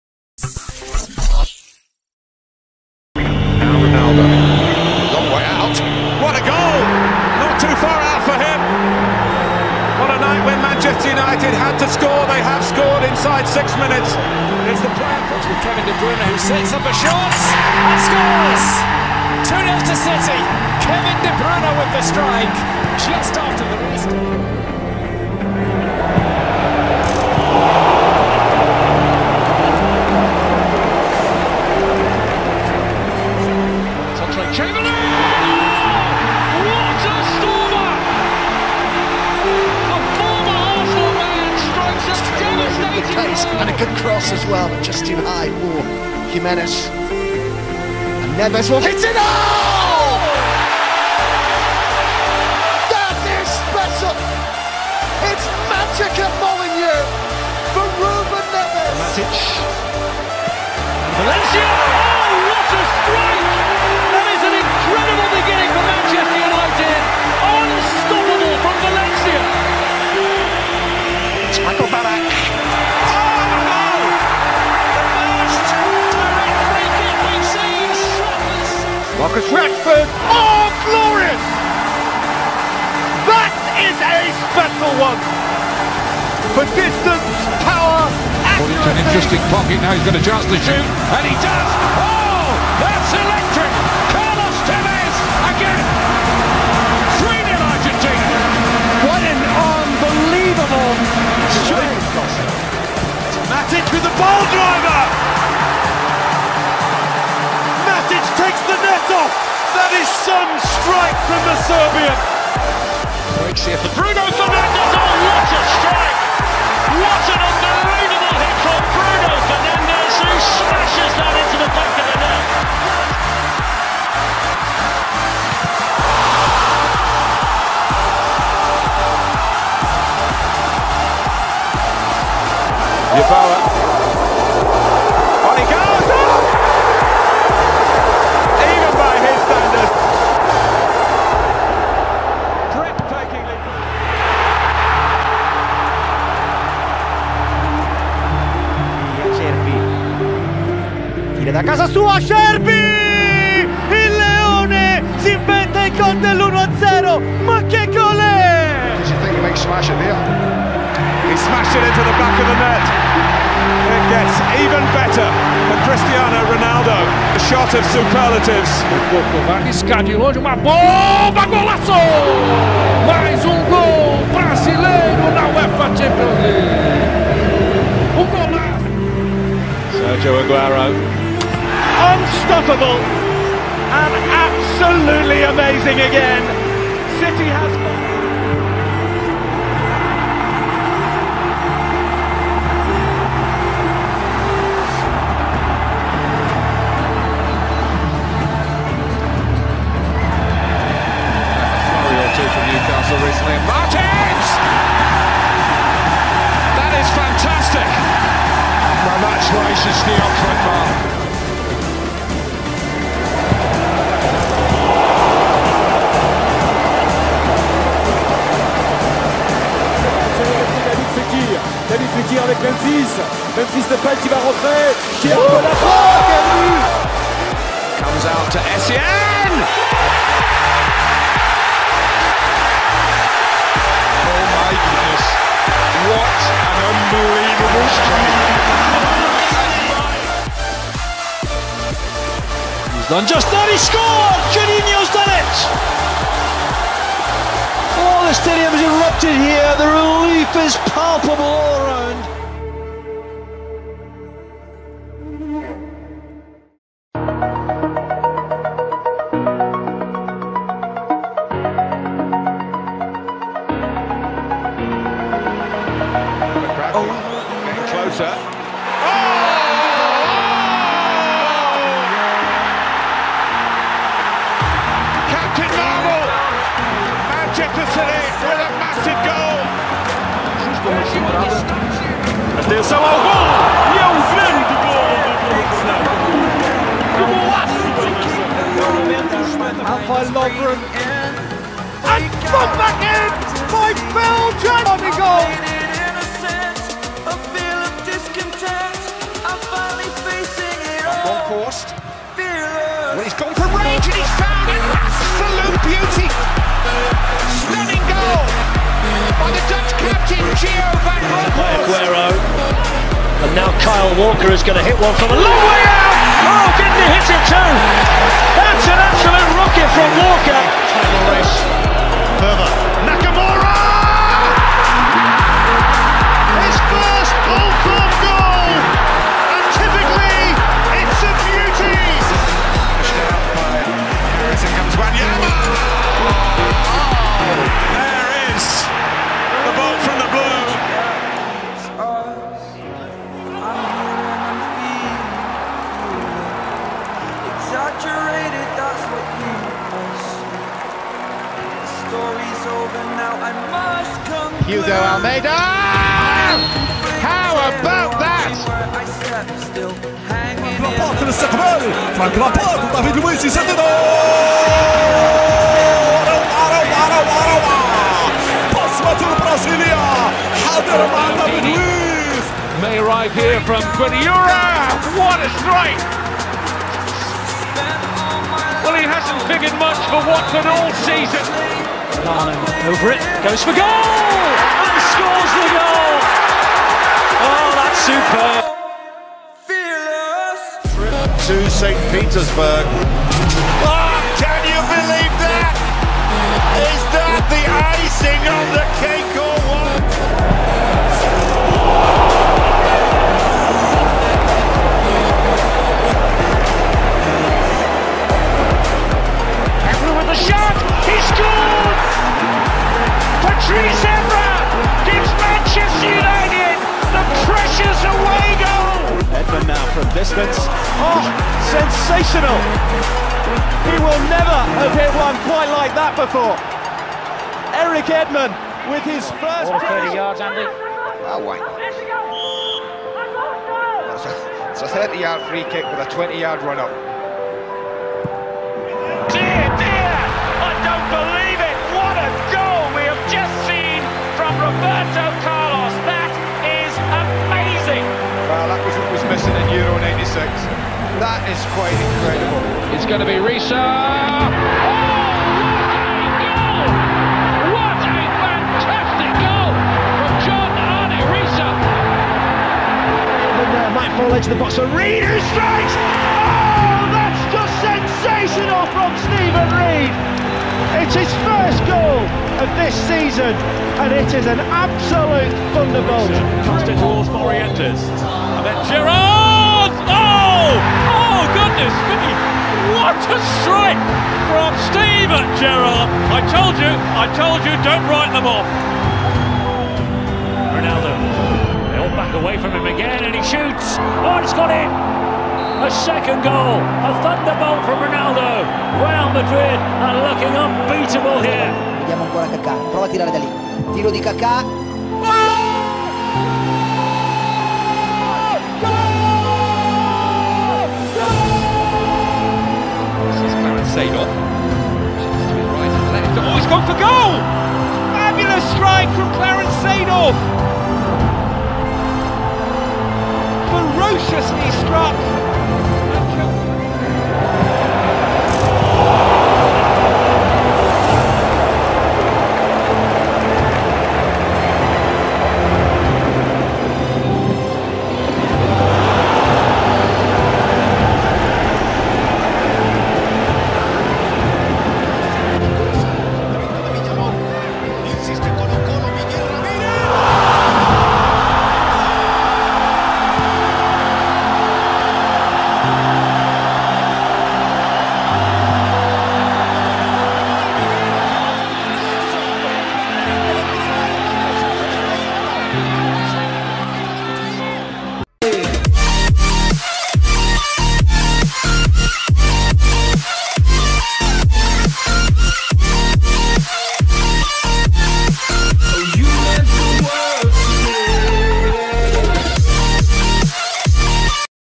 Legendary Rocket Goals In Football 🚀⚽
Get ready to be amazed by these legendary rocket goals in football. From long shots to absolute bangers, this video highlights some of the most impressive and jaw-dropping goals in football history. If you're a fan of the beautiful game, you don't want to miss this!